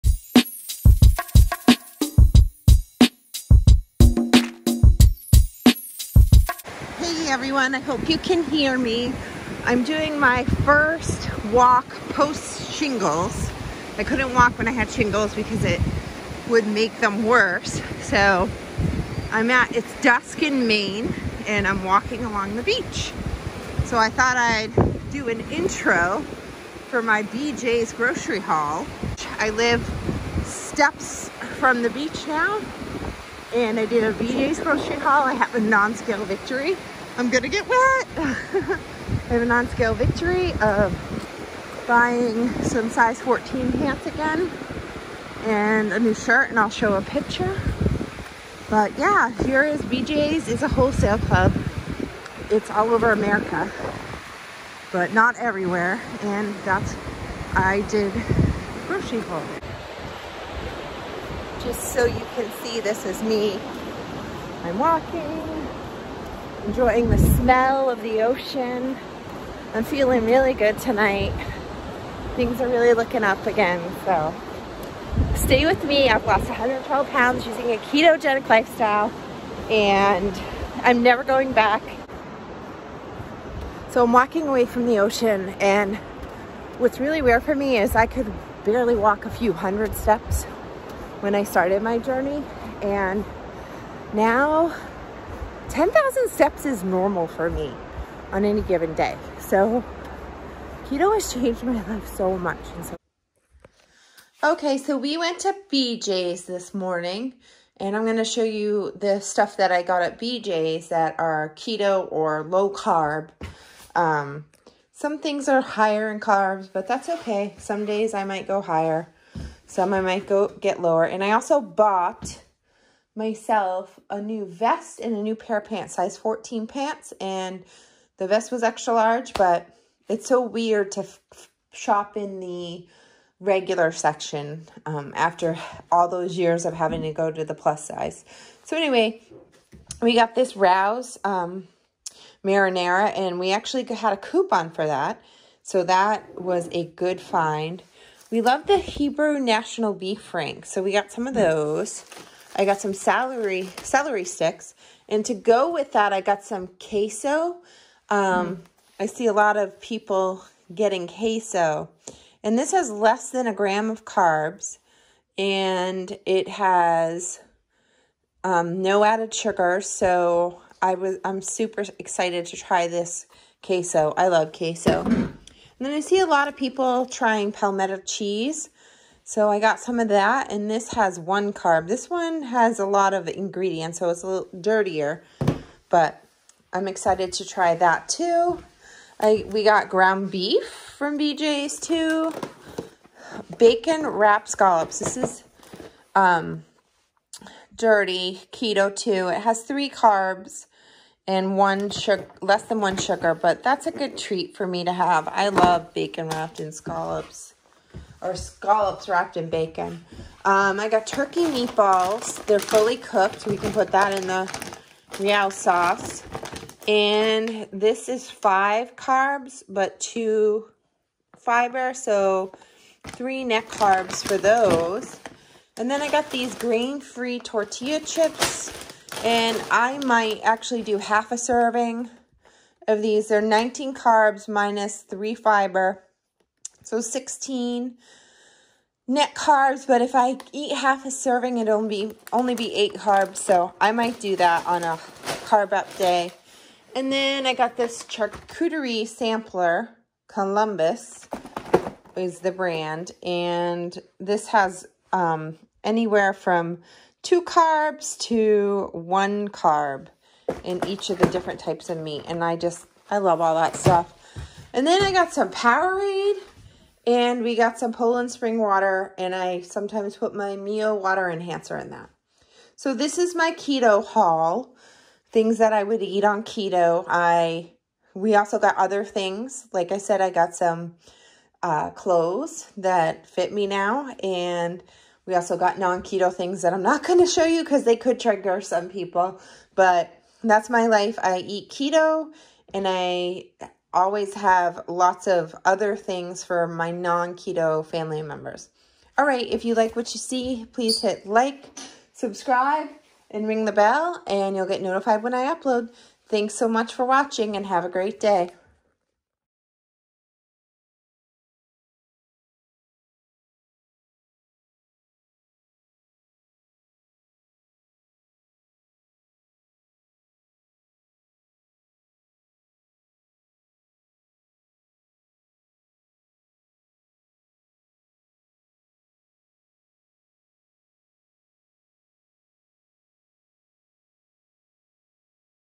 KETO LOW CARB GROCERY HAUL
My keto or low carb grocery haul and a walk on the beach. The haul is from BJ’s wholesale.